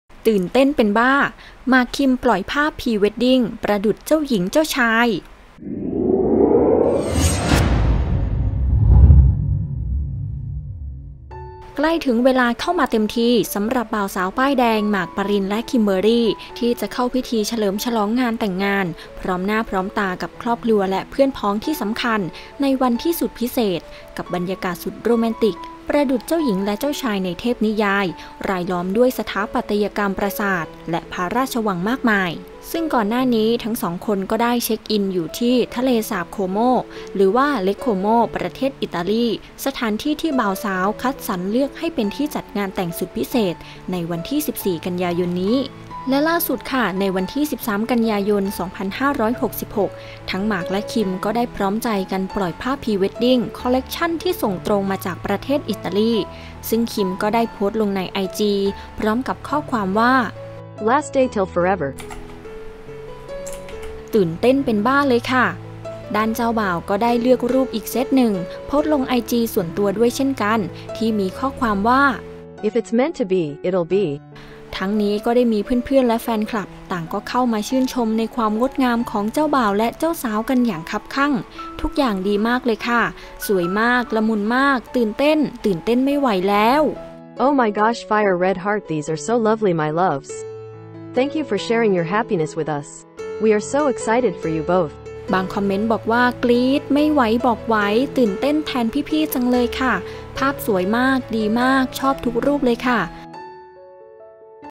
ตื่นเต้นเป็นบ้า! หมาก-คิม ปล่อยภาพพรีเว็ดดิ้ง ประดุจเจ้าหญิง-เจ้าชาย
ใกล้ถึงเวลาเข้ามาเต็มที สำหรับบ่าว-สาว ป้ายแดง ‘หมาก-ปริญ’ และ ‘คิม-คิมเบอร์ลี่‘ ที่จะเข้าพิธีเฉลิมฉลองงานแต่งงาน พร้อมหน้าครอบครัวและเพื่อนพ้องที่สำคัญ ในวันสุดพิเศษ กับบรรยากาศสุดโรแมนติก ประดุจเจ้าชายเจ้าหญิงในเทพนิยาย รายล้อมด้วยสถาปัตยกรรมปราสาทและพระราชวังมากมาย